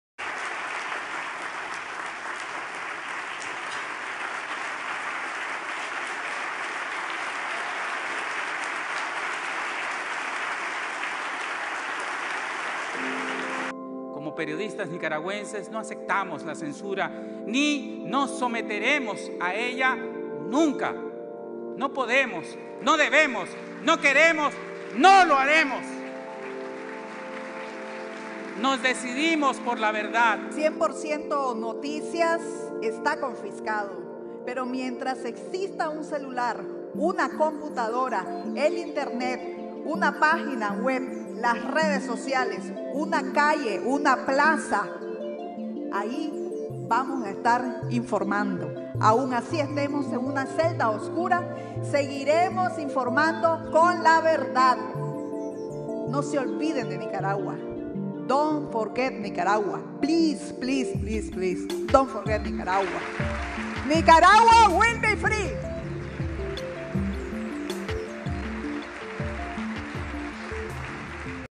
Periodistas Lucía Pineda y Miguel Mora reciben Premio Internacional a la Libertad de Prensa 2019
Los exreos políticos y periodistas nicaragüenses Miguel Mora y Lucía Pineda Ubau, recibieron la noche del jueves El Premio Internacional de Libertad de Prensa, otorgado por el Comité para la Protección de los Periodistas (CPJ, por sus siglas en inglés), en Nueva York, Estados Unidos.